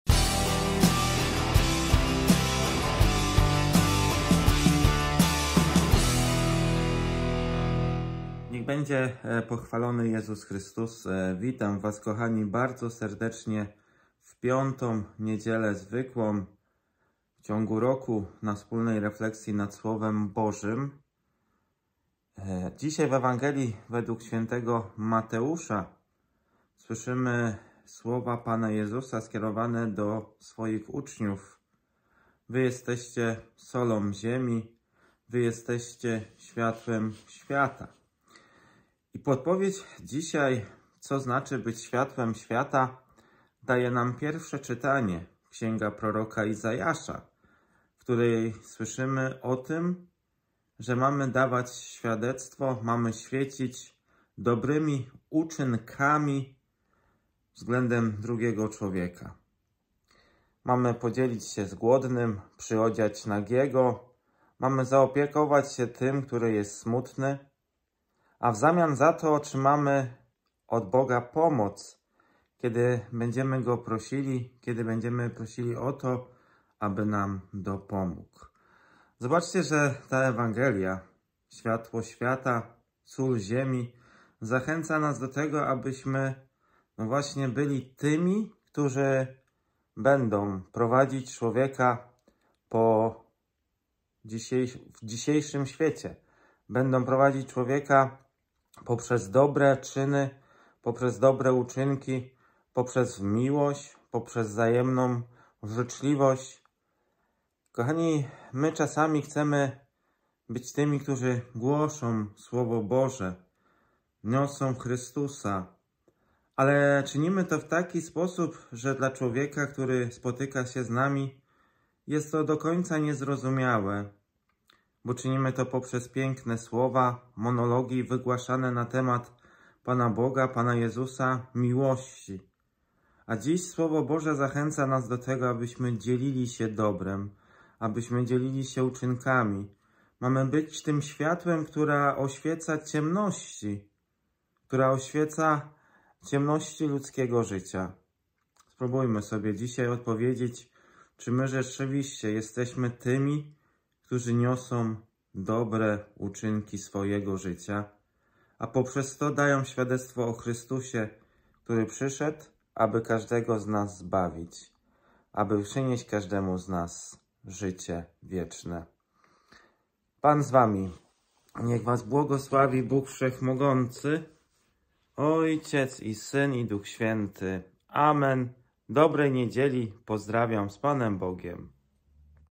Ewangelia Życia - Słowo Boże na każdy dzień - niedziela 5 II 2023
Komentarz do Ewangelii według Świętego Mateusza 5, 13 - 16,